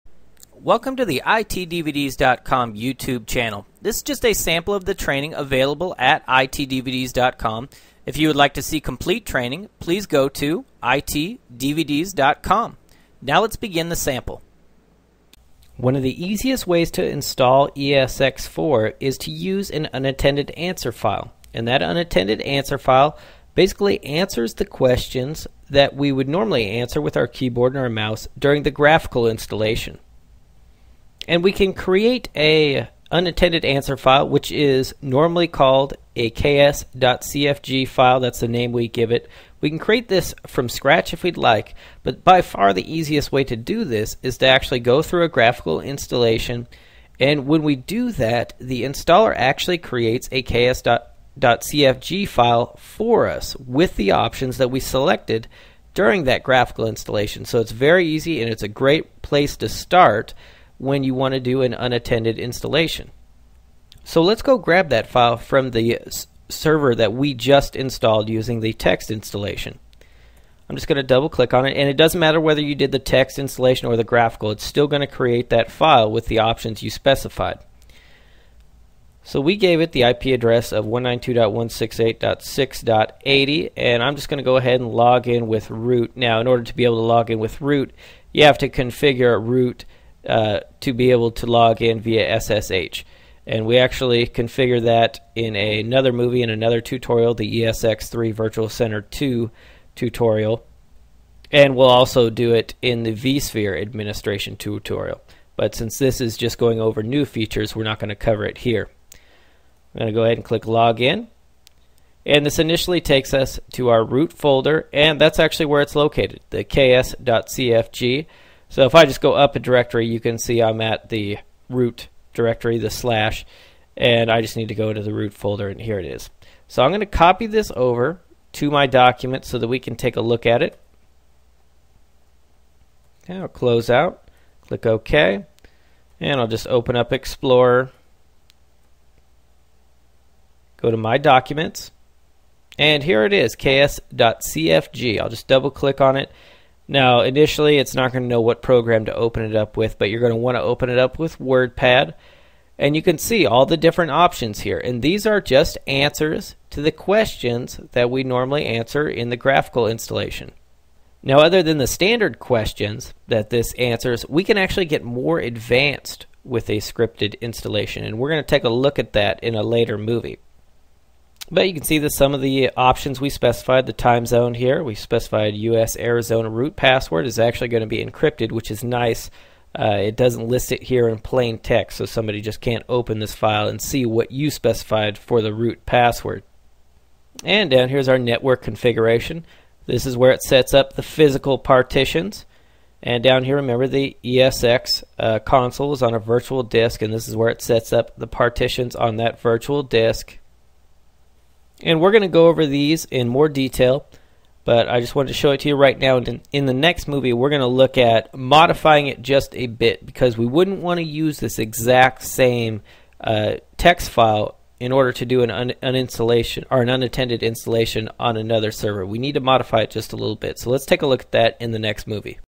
VMware vSphere CBT - Obtaining a Preconfigured ks.cfg Unattended Script File
VMware vSphere Administration Training by ITDVDS.com that covers ESX 4 and vCenter 4.  This is just a sample from the vSphere Administration training by ITDVDS.com that goes over Obtaining a Preconfigured ks.cfg Unattended Script File for ESX 4 automated installation.